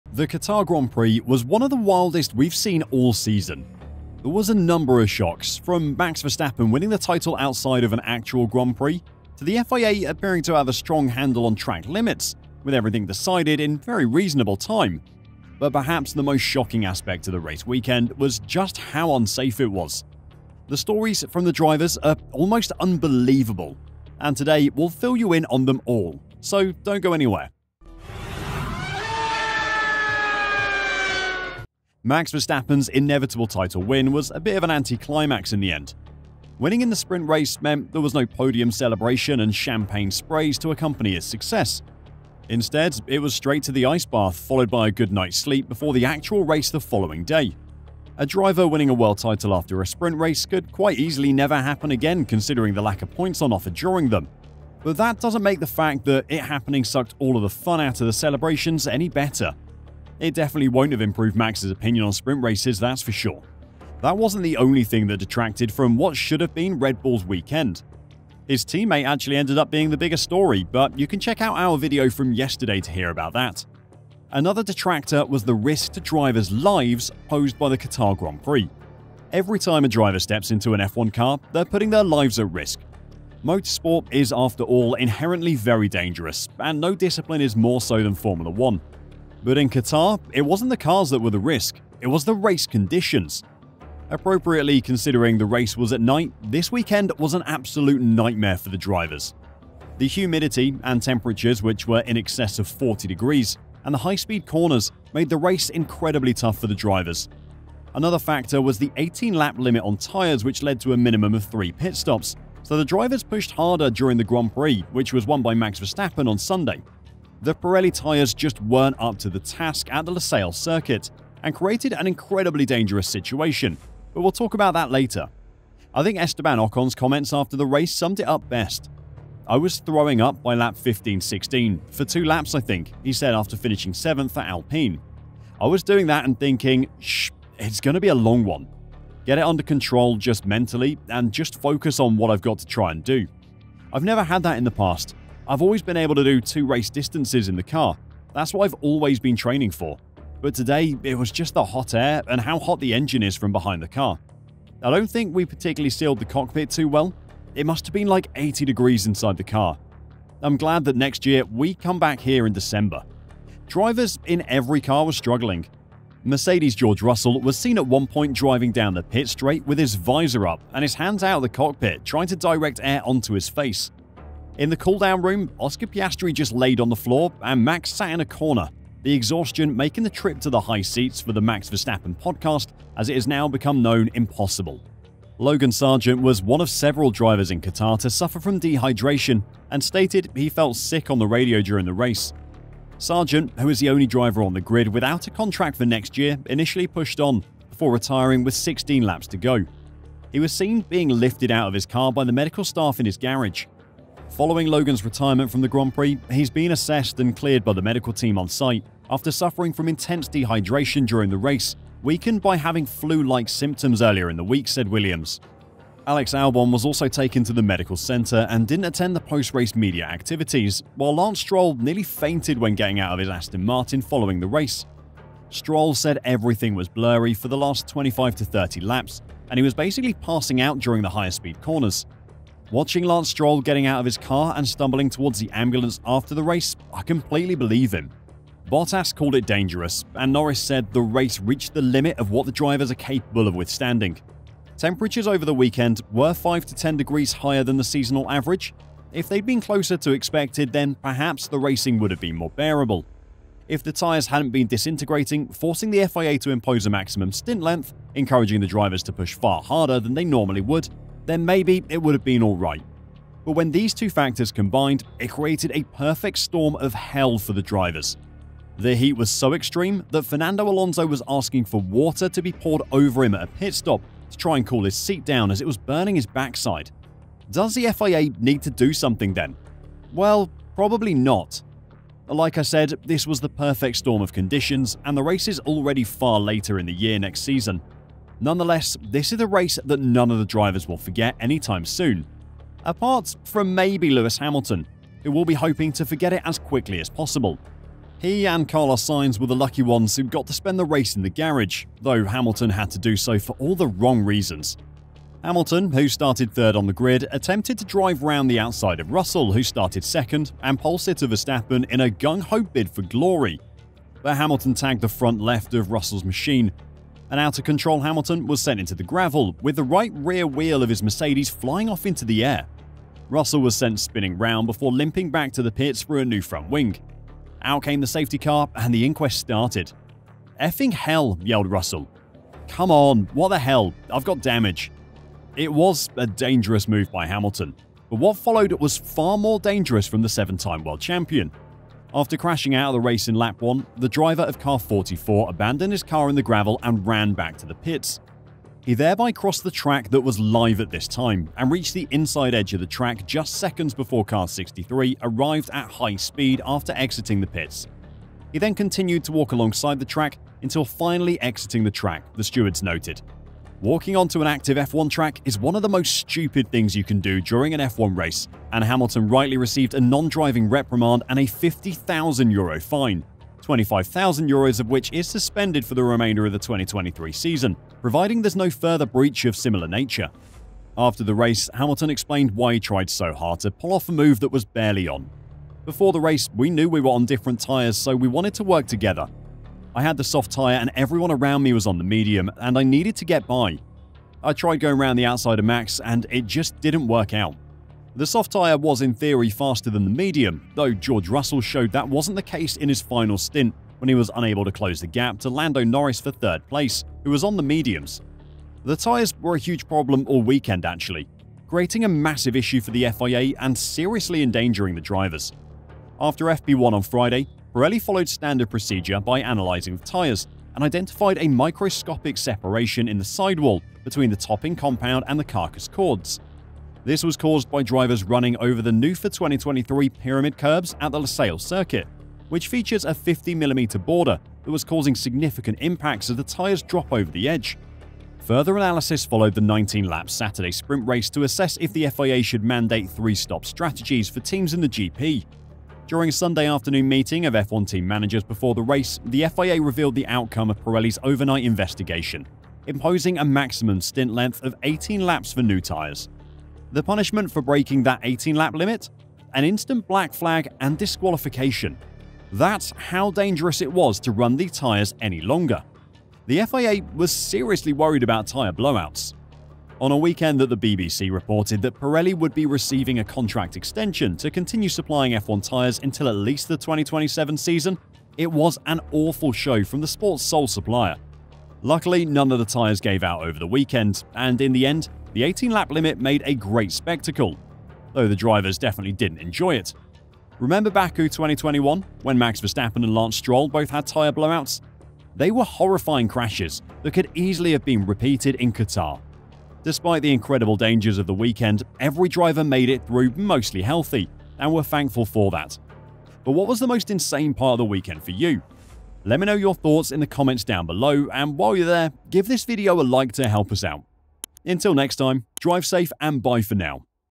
The Qatar GP was BRUTALLY DANGEROUS!!
The Qatar Grand Prix was one of the wildest we have seen all season. There were a number of shocks from Max Verstappen winning the title outside of an actual Grand Prix, to the FIA appearing to have a strong handle on track limits, with everything decided in very reasonable time. But perhaps the most shocking aspect of the race weekend was just how unsafe it was. The stories from the drivers are almost unbelievable and today, we’ll fill you in on them all, so don’t go anywhere.